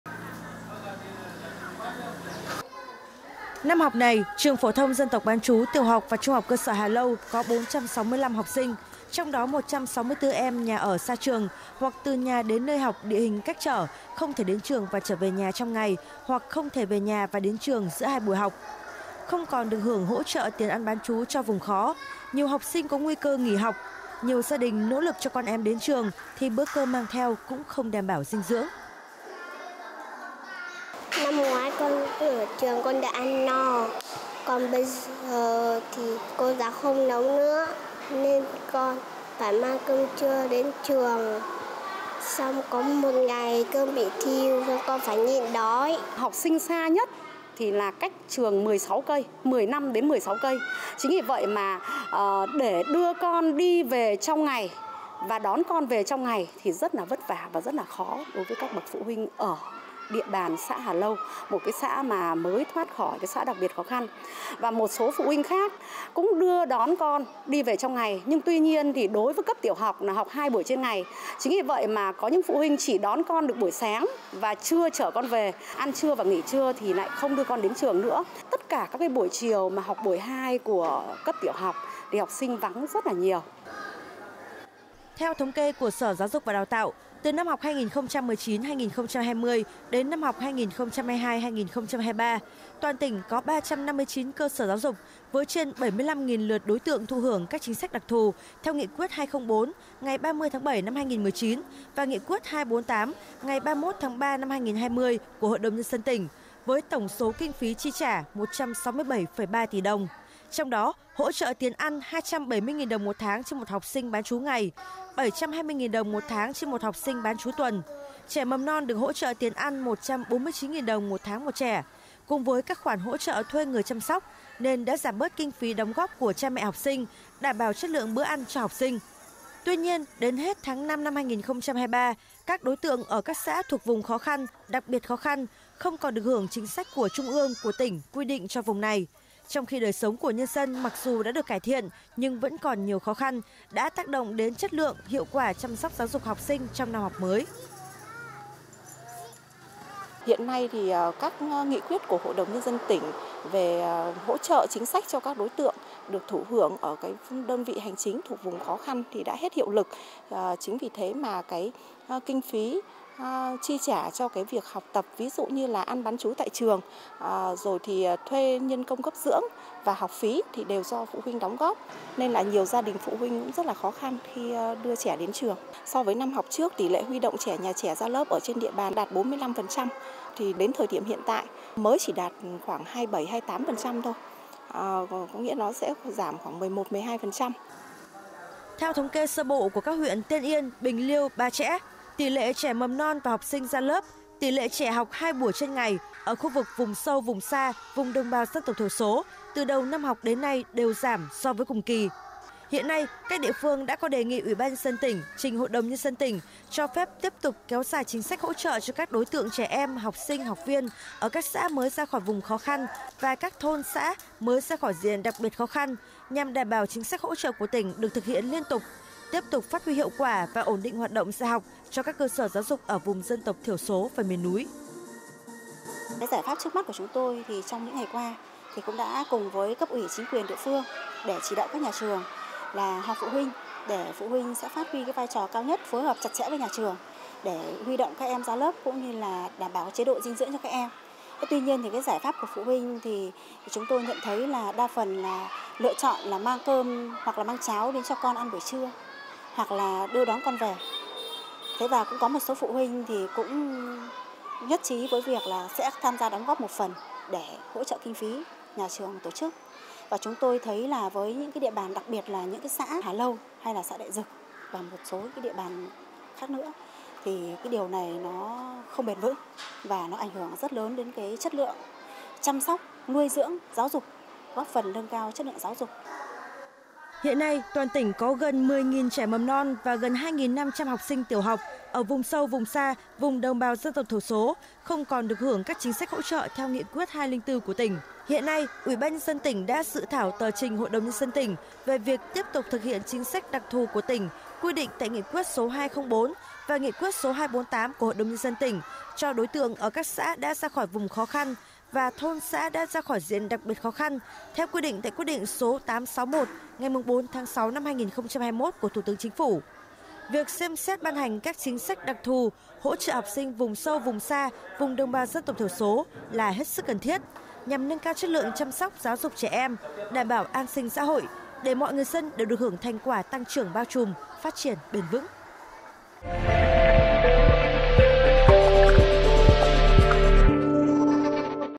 Xem xét ban hành chính sách hỗ trợ giáo dục vùng cao
Với học sinh dân tộc, vùng sâu, vùng xa, những bữa ăn bán trú trở thành giải pháp hữu hiệu góp phần duy trì tỷ lệ chuyên cần và nâng cao chất lượng giáo dục. Tuy nhiên, kết thúc năm học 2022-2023 vừa qua, các chính sách đặc thù cho giáo dục vùng cao, trong đó có hỗ trợ tiền ăn cho trẻ mầm non và học sinh bán trú ngày, bán trú tuần theo Nghị quyết 204 ngày 30/7/2019 của HĐND tỉnh đã hết thời gian thực hiện.